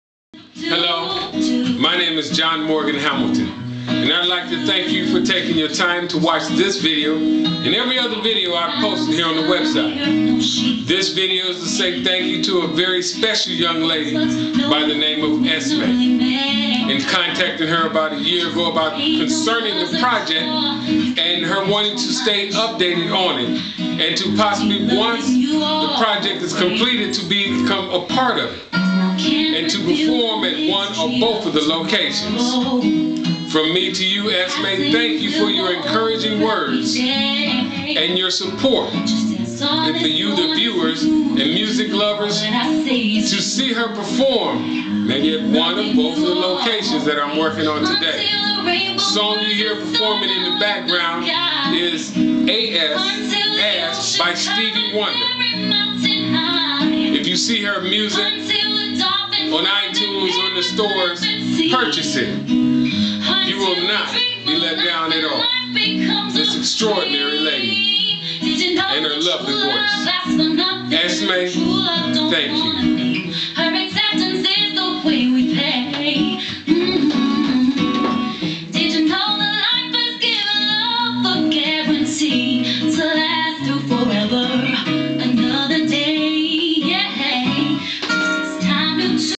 ESMEE - DREAMS TO REALITY SHOUT OUT
THANK YOU ESMEE FOR YOU SUPPORT, I MEANS A LOT TO ME. ONE OF MANY ENTERTAINERS TO POSSIBLE PERFORM AT THE PORJECT